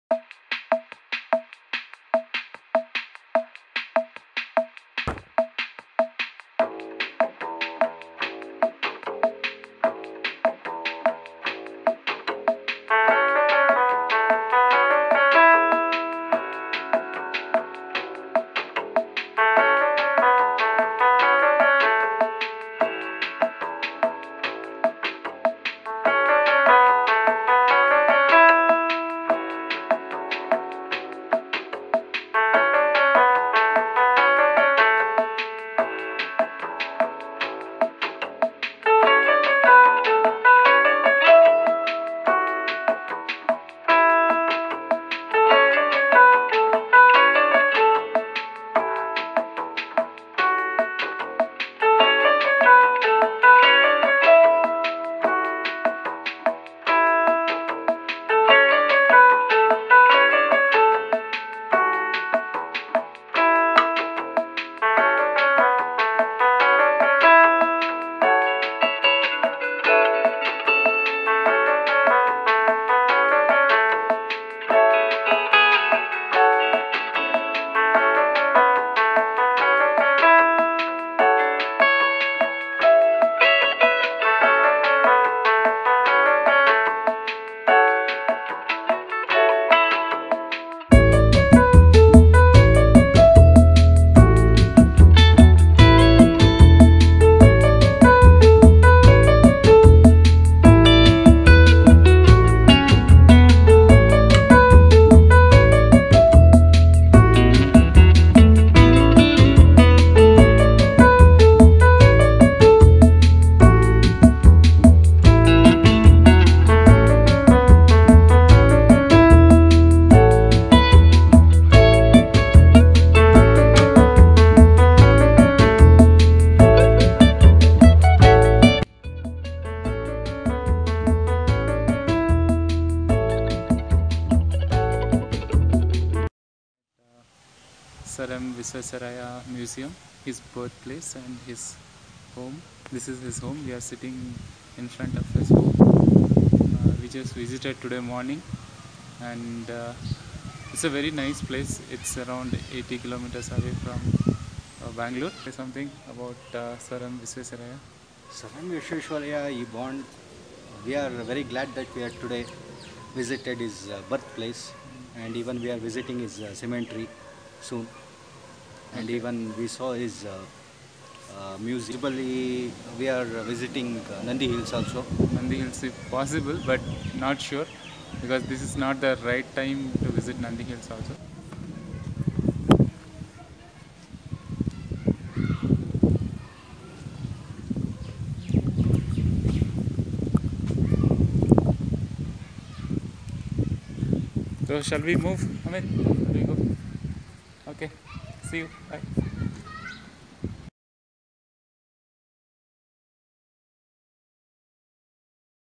Sir M Visvesvaraya birth place (Bangalore to Muddenahalli)
SIr Mokshagundam Visvesvaraya  was an Indian engineer.15th September his birth date celebrated as engineers's day in India. He is a recipient of the Indian Republic's highest honour, the Bharat Ratna.